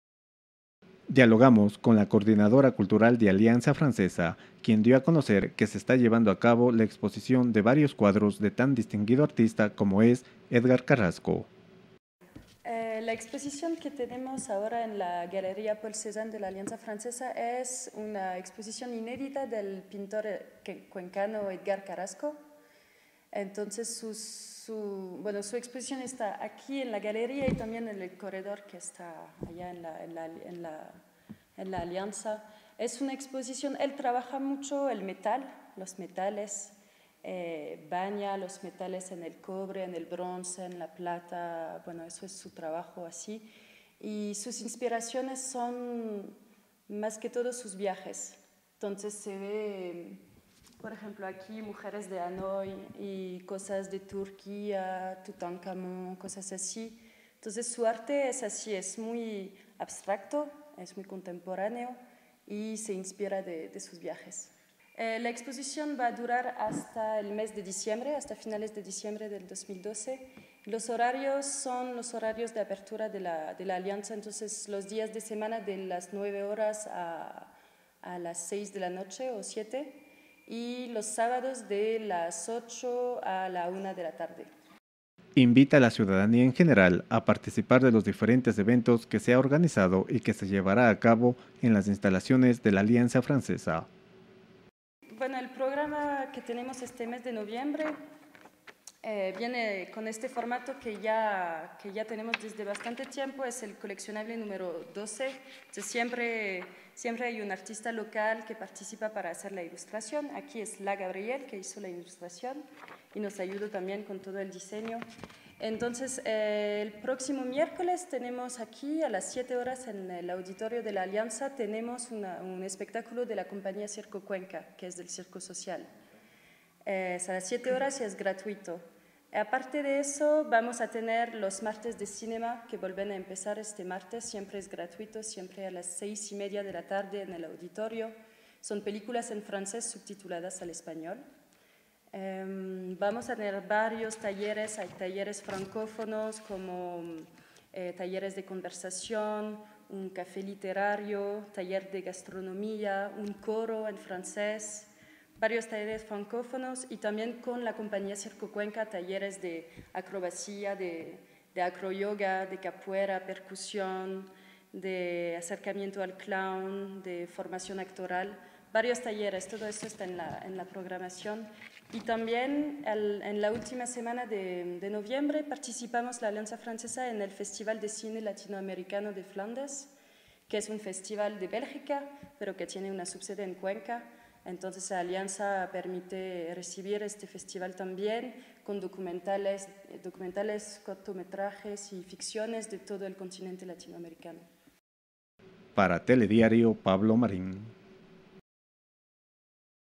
ALIANZA FRANCESA REALIZA UNA EXPOSICION CON CUADROS DE ARTISTA CUENCANO .m4v
ALIANZA FRANCESA  invita a la exposición de las obras del afamado artista Edgar Carrasco.